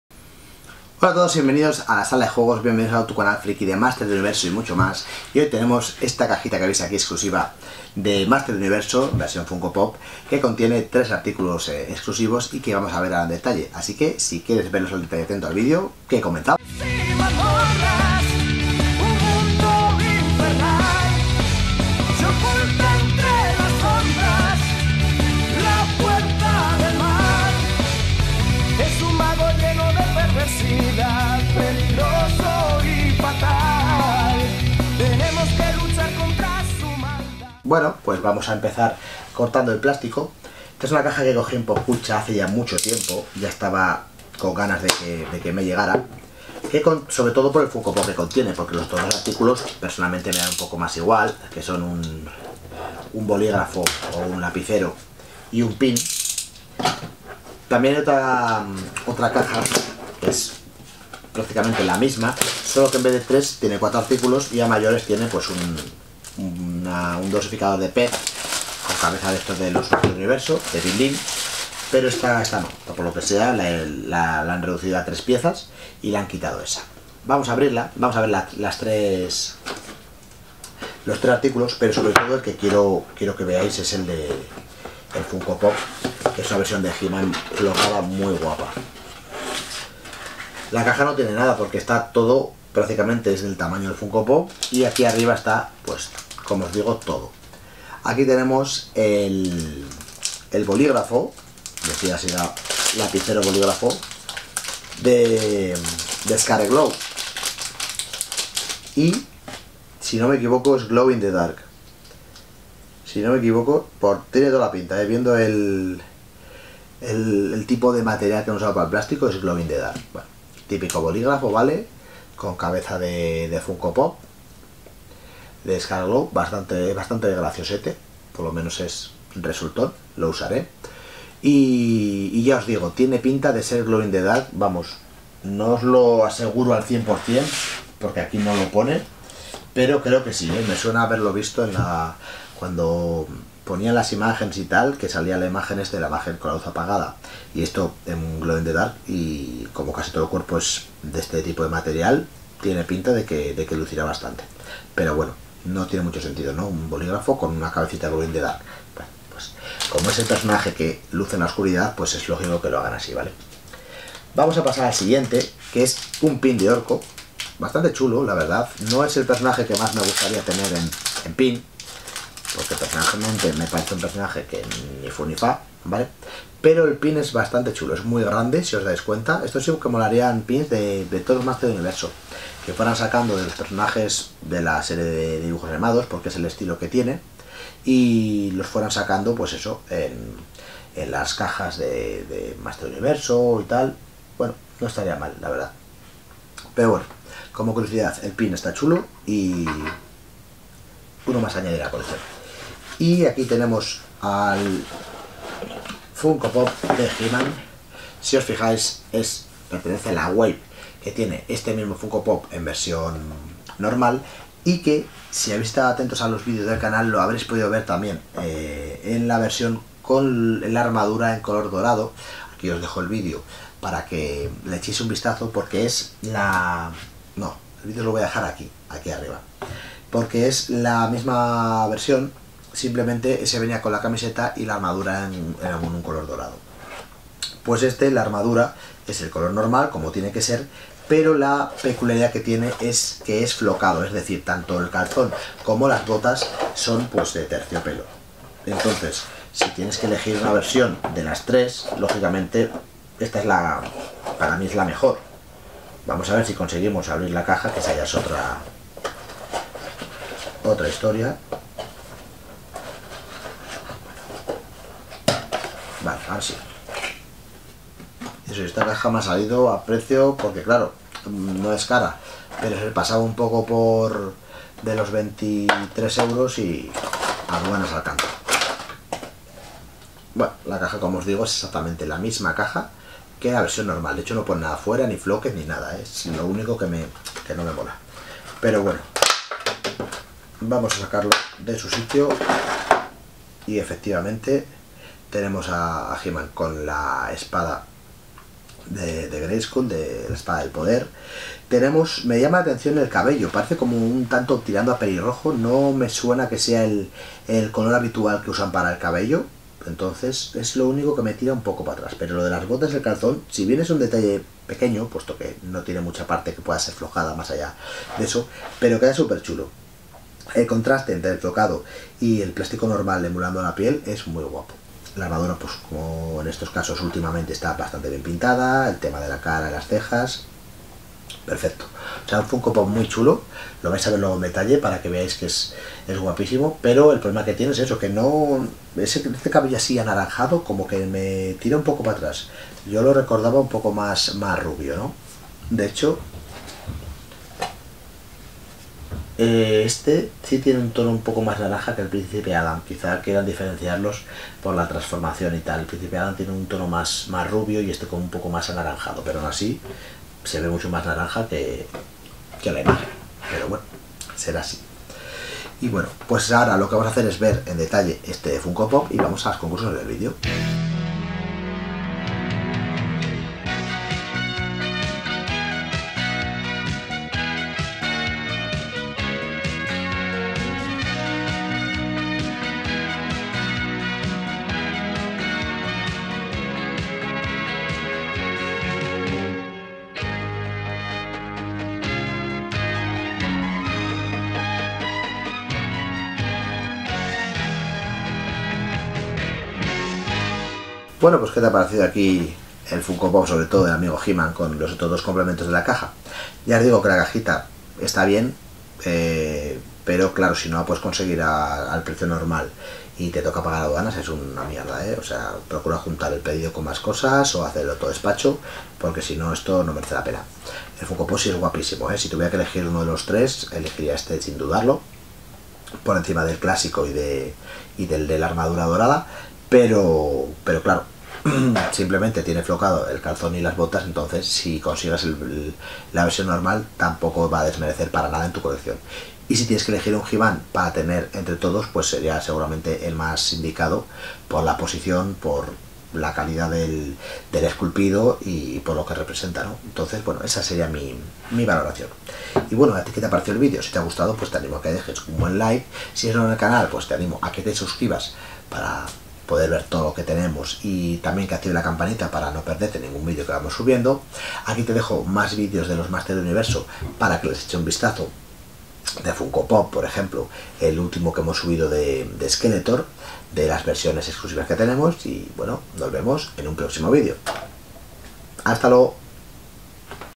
CAJA temática Funko Pop 💪 MASTERS DEL UNIVERSO 💪 - ¡He-Man Flocked!
Hoy os traemos al canal el unboxing de la caja exclusiva de Game Stop que salió fuera de USA de temática de los Masters del Universo para Funko.

Muchas gracias y nos vemos en el siguiente vídeo